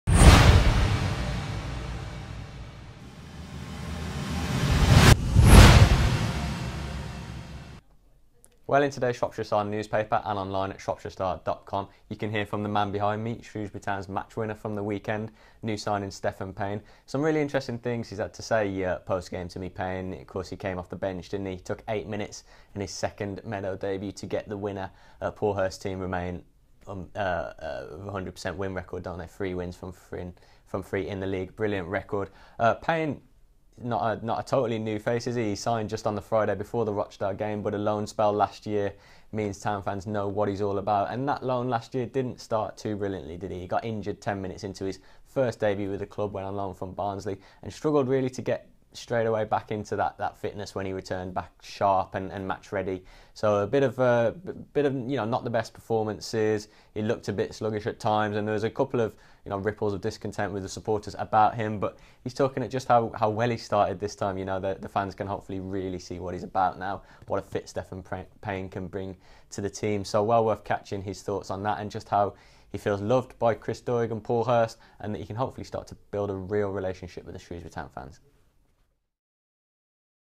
Lewis Cox's Shrewsbury Town update - August 22
The Shropshire Star's Lewis Cox on the latest from Shrewsbury Town where Stefan Payne made a dream debut against Rochdale.